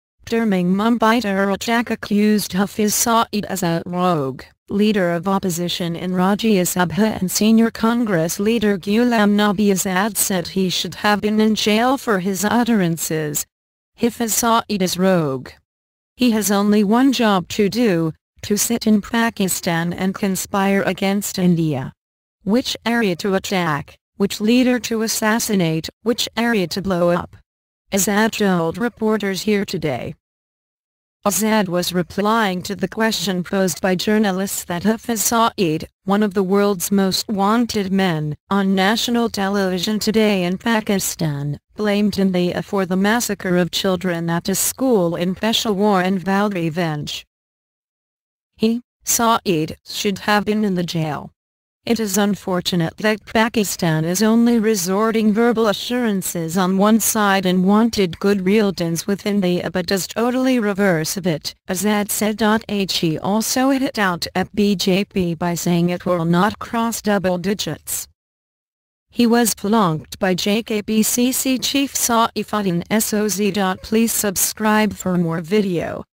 Hafiz Saeed a rougue ` should have been in jail Azad a report
Terming Mumbai terror attack accused Hafiz Saeed as a "rogue", Leader of Opposition in Rajya Sabha and senior Congress leader Gulam Nabi Azad said he should have been in jail for his utterances."Hafiz Saeed is rogue. He has only one job to do - to sit in Pakistan and conspire against India. Which area to attack, which leader to assassinate, which area to blowup...," Azad told reporters here today.

Azad was replying to the question posed by journalists that Hafiz Saeed, one of the world's most-wanted men, on national television today in Pakistan, blamed India for the massacre of children at a school in Peshawar and vowed revenge.

"He (Saeed) should have been in the jail. It is unfortunate that Pakistan is only resorting verbal assurances on one side and wanted good realtions with India but does totally reverse of it," Azad said.He also hit out at BJP by saying it will not cross \

double digits.